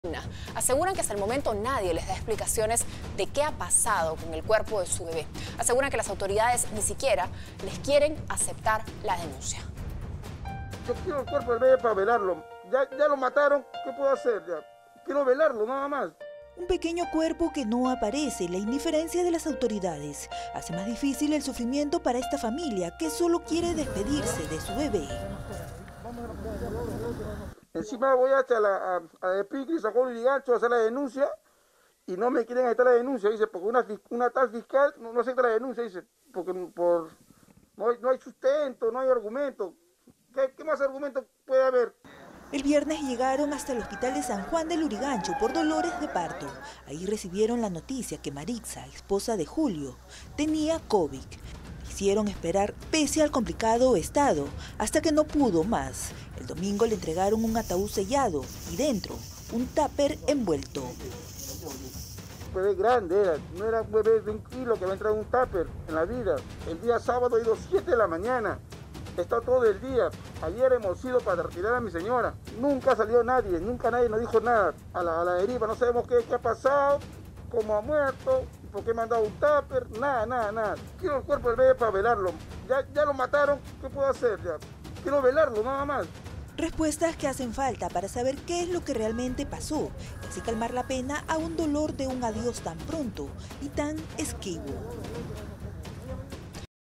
Familia denunció que hospital les dio un táper en lugar del cuerpo de su bebé fallecido
Cuentan que se percataron de la ausencia del cadáver cuando abrieron el pequeño ataúd.